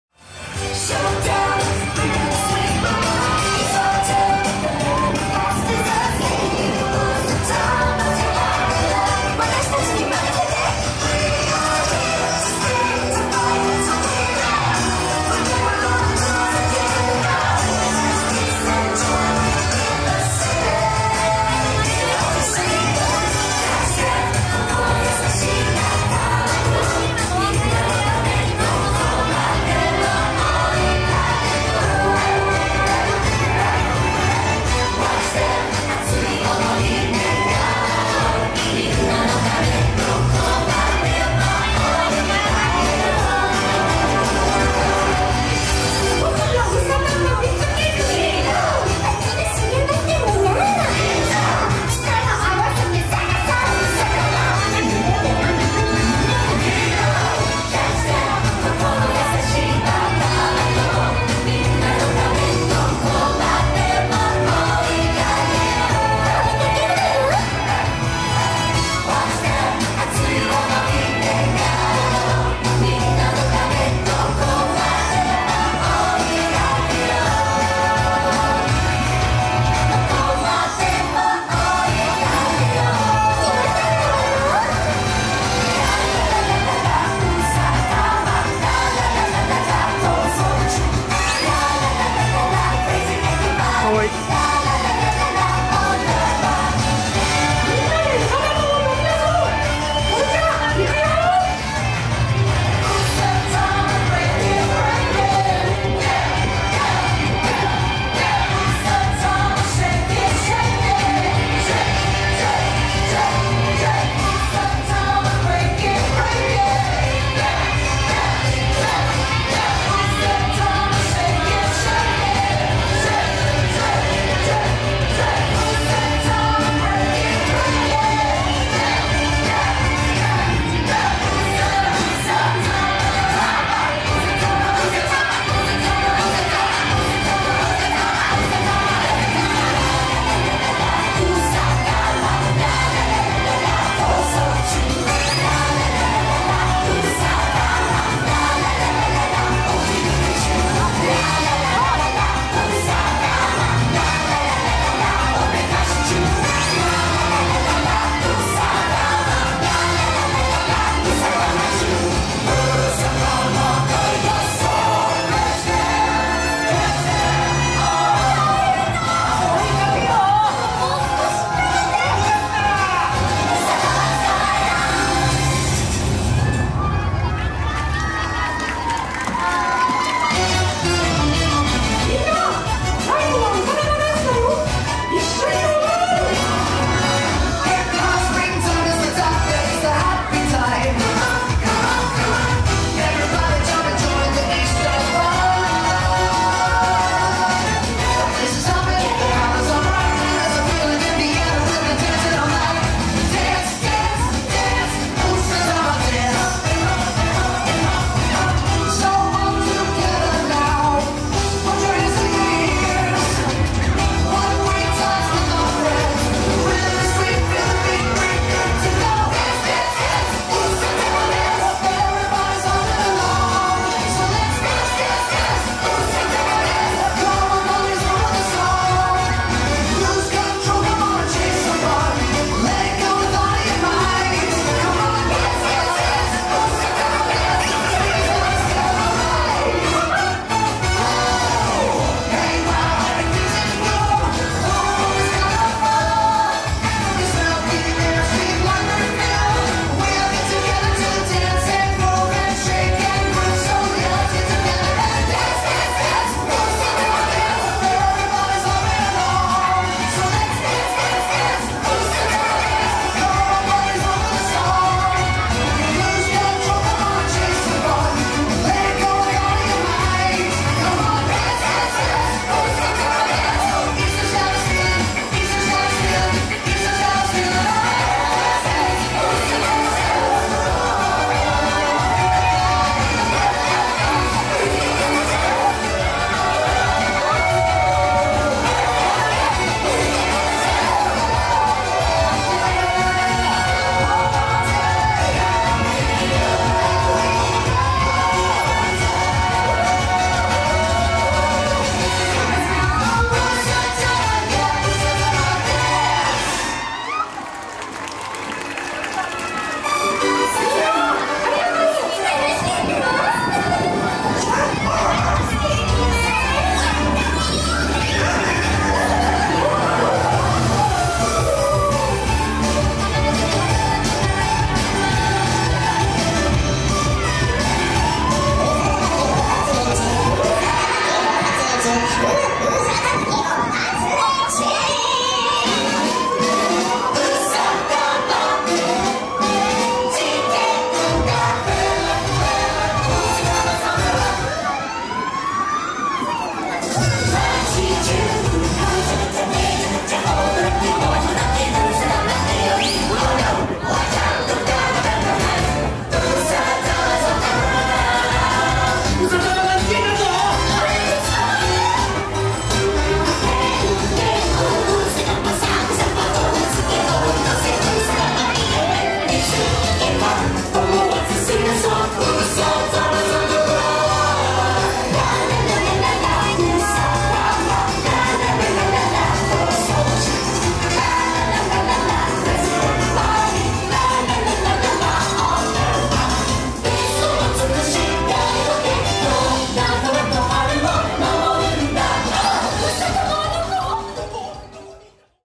【初日・ミニー・高画質】うさたま大脱走！（2019年4月4日 TDL）
今年で 2年目となりましたディズニーランドのイースター・パレード「うさたま大脱走！」の初日、2ステの動画です。ミニーさんの乗ったフロートが停止する 1stポジから撮りました。ちょうどカンベア（カントリーベア・シアター）前の立ち見エリアからです。

こちらもユニバーサル・ミュージック・グループより YouTube での動画公開がブロックされていましたが、CD音源の一般発売に伴いブロックが解禁されました。ちなみにブロック中は当方の拙いトークによる副音声動画を公開しておりました（

さて、2019年3月26日より東京ディズニーリゾートにおけるミッキー及びミニーが、グローバル・スタンダードであるニュールックへと変更されました。当初ヲタども…と申しますか当方の勝手な予想としましては、「既存のショーパレは変更無いんじゃね？」とか思っていたのですが、その期待は見事に裏切られ、キレイに、隅から隅までずずずいーっと、一ミリも余すこと無くニュールックへと変更になりました。一部、ホテルでの挙式は来年 2020年までは旧ルックとのことです。

ということで、ニュールックなうさたま初披露ということです。まぁ確かにミニーさんはカワイイ。うん、カワイイ。でも、やっぱりカワイイに寄ってしまって「美しさ」が無くなりました。これはこれでヲタとしては受け入れていくべきなのか…。色々と複雑な心境ではありますが…。